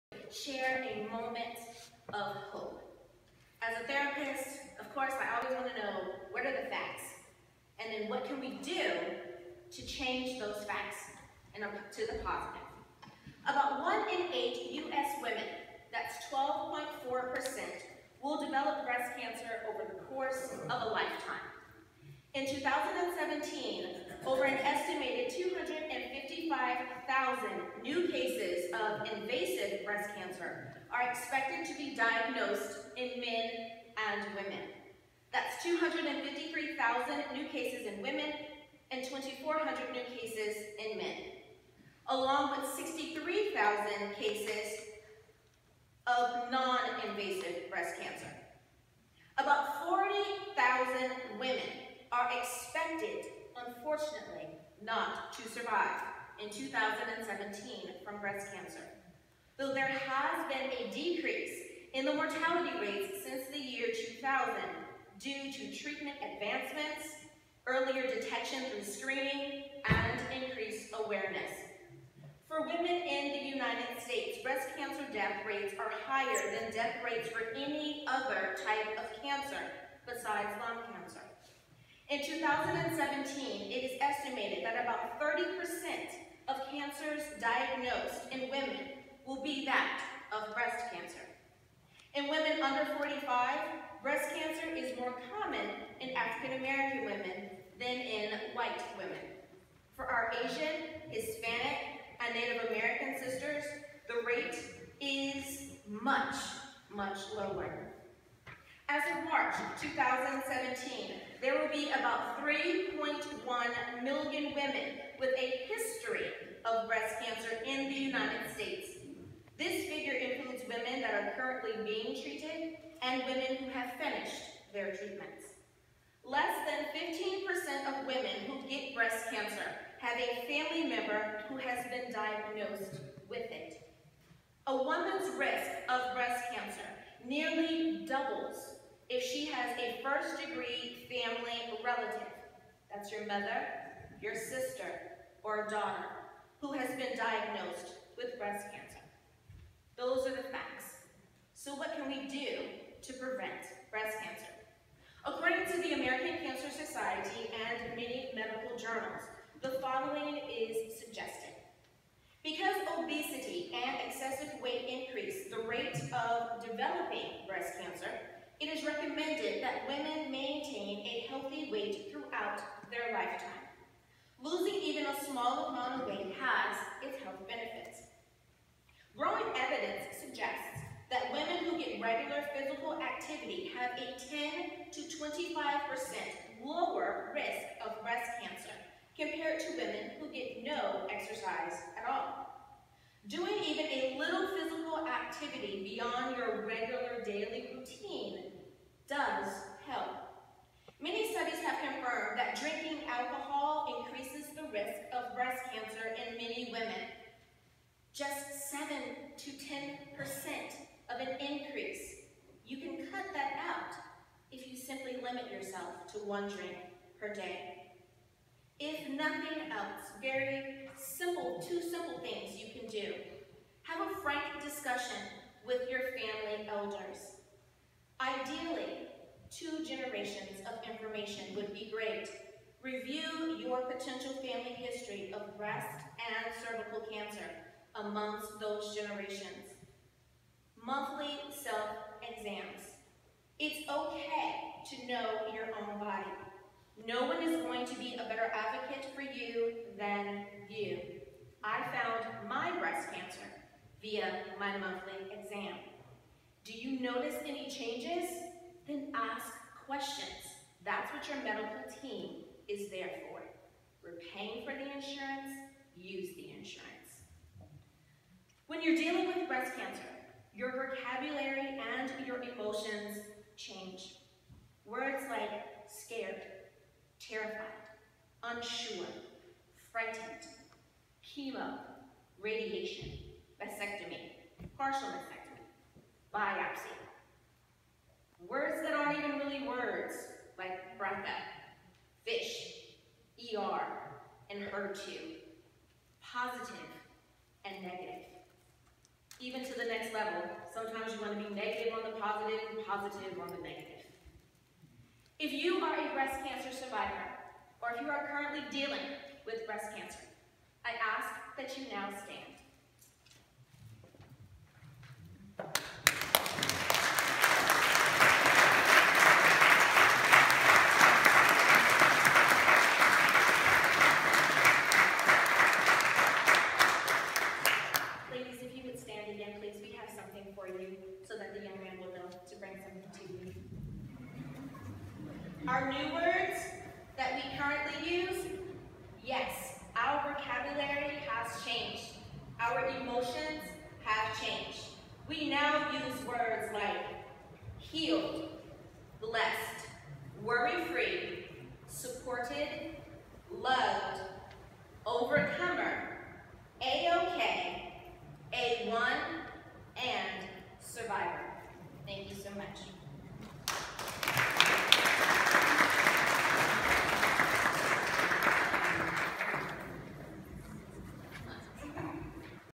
A Moment of Hope
Breast Cancer awareness at Carver Memorial Presbyterian Church. October 29, 2017.

Dr. Angela Butts Chester
Pastoral Counselor | Minister | Motivational Speaker | Radio Host